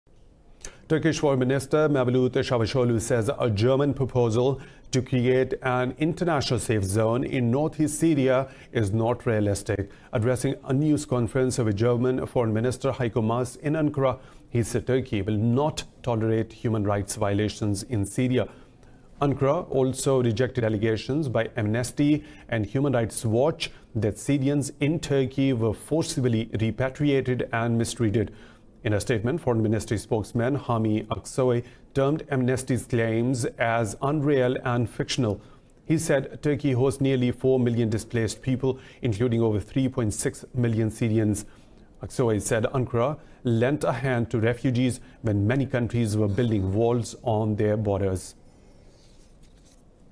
Turkey: Germany’s Safe Zone Proposal Unrealistic | Indus News
Addressing a news conference with German foreign minister HeikoMaas in Ankara, Turkish Foreign Minister MevlutCavusoglu says a German proposal to create an international safezone in northeast Syria is not realistic.

Indus News Live: youtu.be/_HXC3vnd4SQ